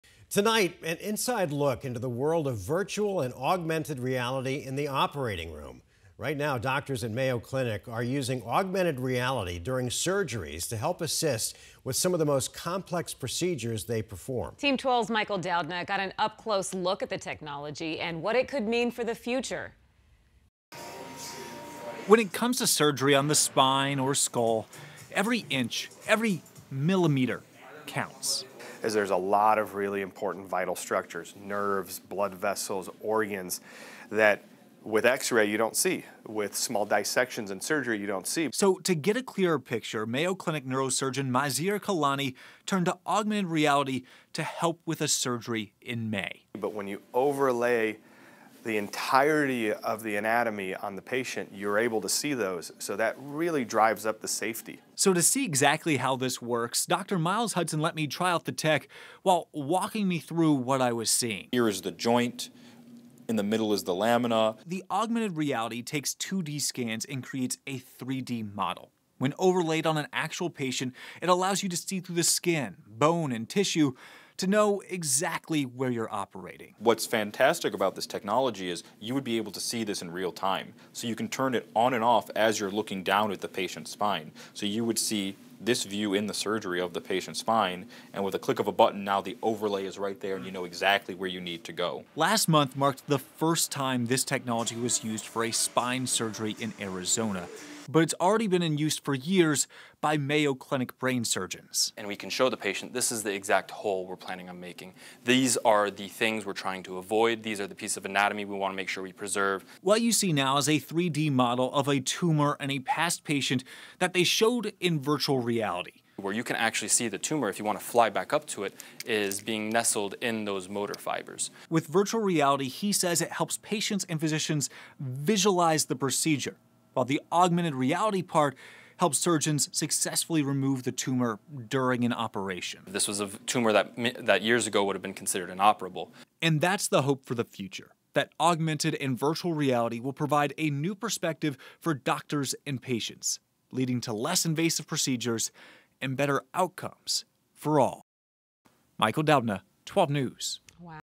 How Mayo Clinic uses augmented reality in the operating room
12News got a hands-on experience with the Mayo Clinic's augmented reality technology, which is used by doctors for complex medical procedures.